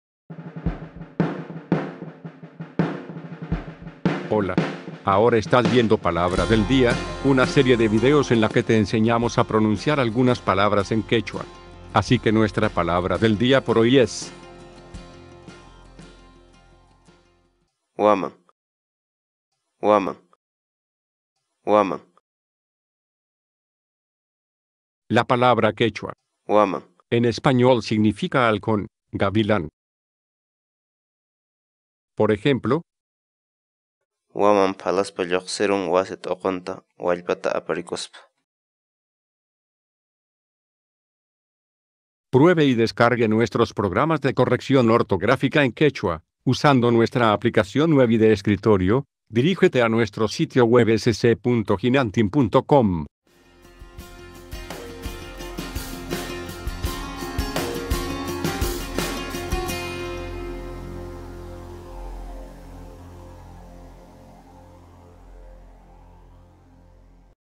Como pronunciar waman ‘halcón, gavilán’
Social

Grupo de Investigación Hinantin: La entrada ‘waman’ es parte de la base para el desarrollo de un diccionario hablado disponible gratuitamente para el idioma quechua.